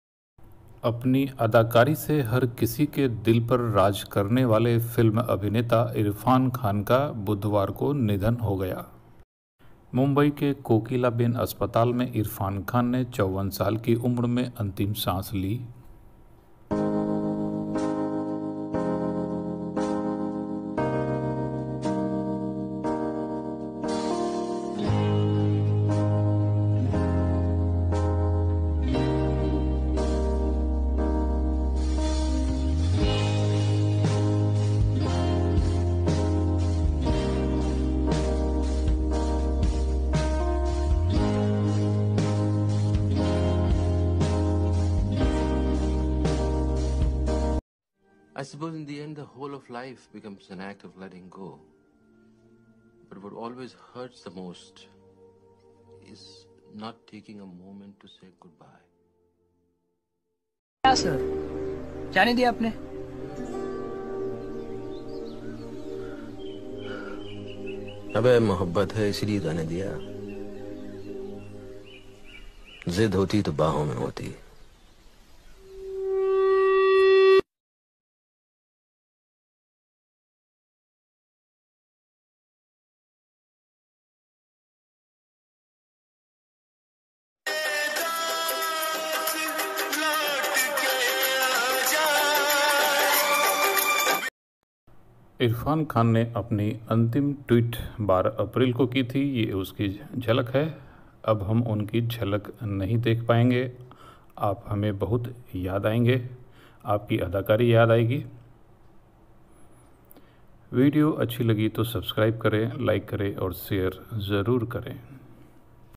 Irfan Khan A magnificent actor dies at 53।इरफान खान का निधन।Irfan's Last tweet।Omg Bihari।
इस वीडियो में Irfan Khan A magnificent actor dies at 53।इरफान खान का निधन।Irfan's Last tweet। 2018 में बीमार पड़े कोलोन कैंसर से पीड़ित इरफ़ान खान का निधन कोकिलाबेन अस्पताल मुम्बई में हो गया। अदाकारी के धनी Irfan Khan दो साल से बीमार थे।